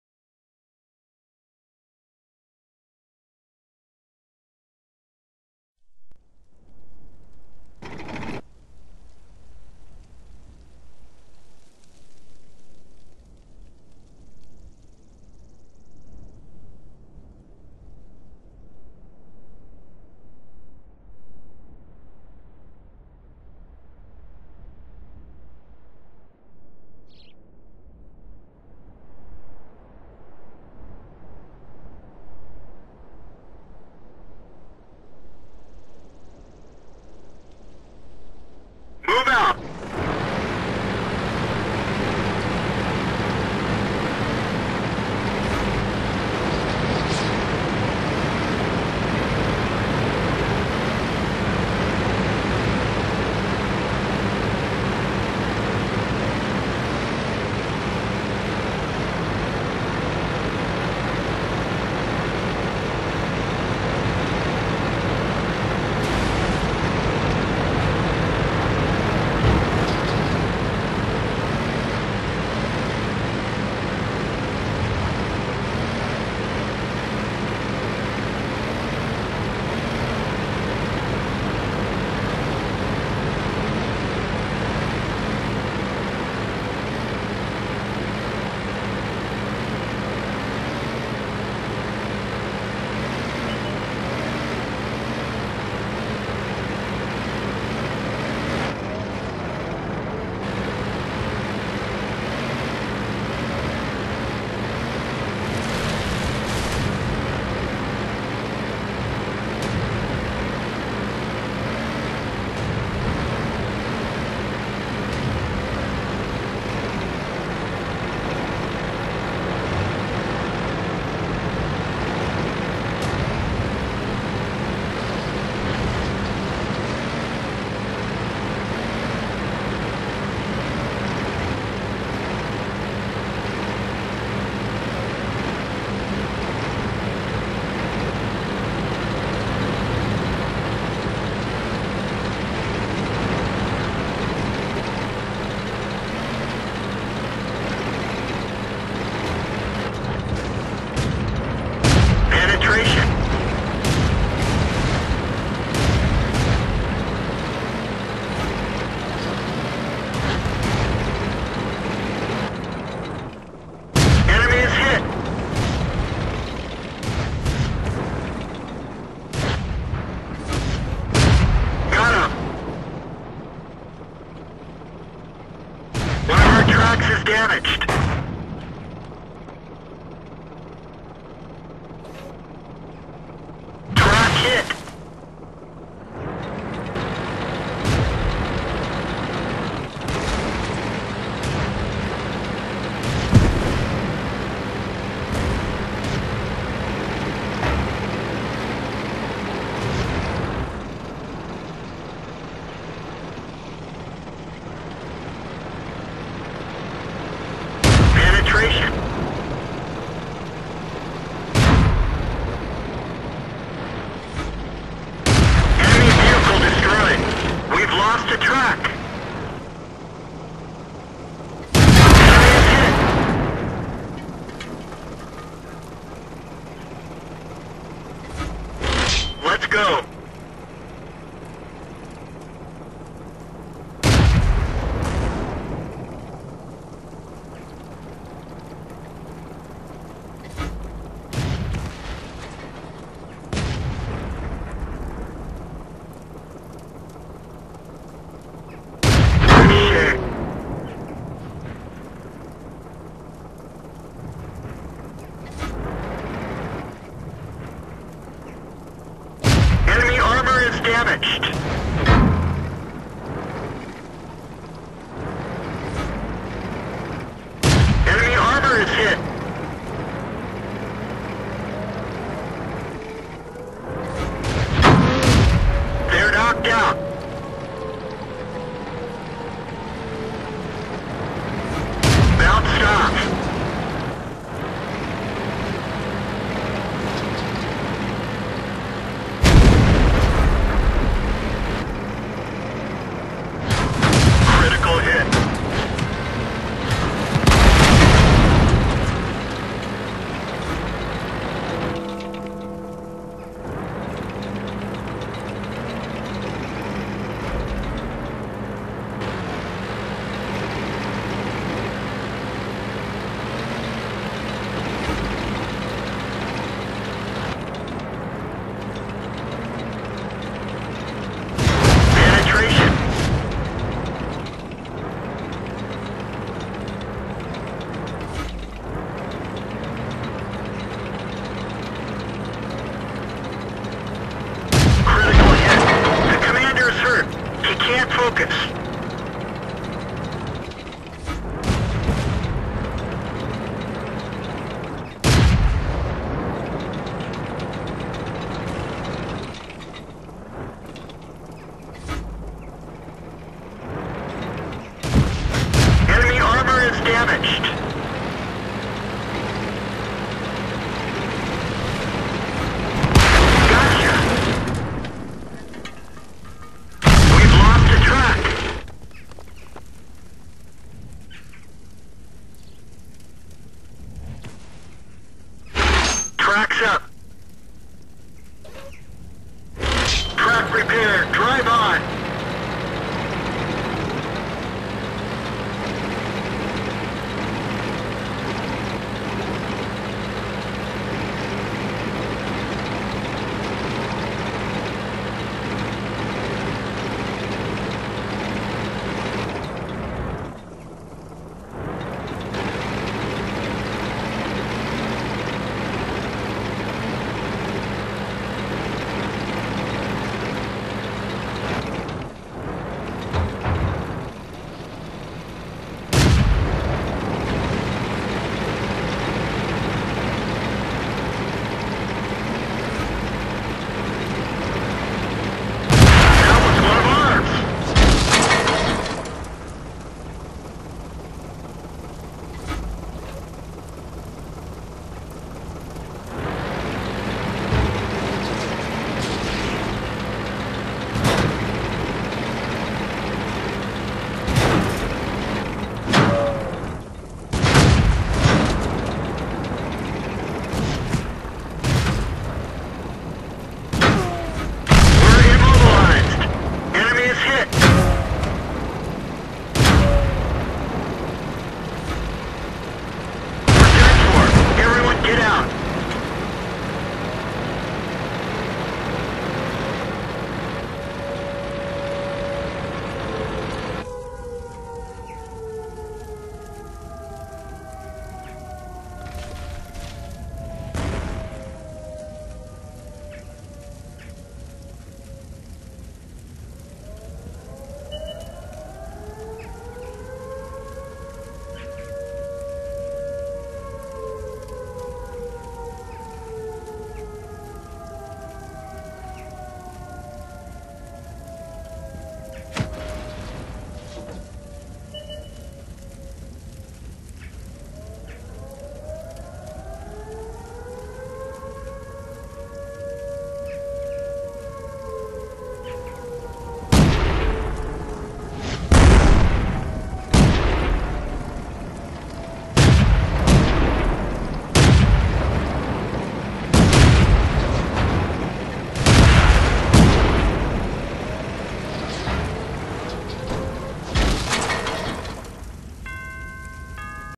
AMX 40: A painful lesson in situational awareness
In this video, the game starts out great, and continues that way, until I mess it up for the whole team.  Let this be a lesson to all: pay attention to your team's positions, or this could happen to you, too.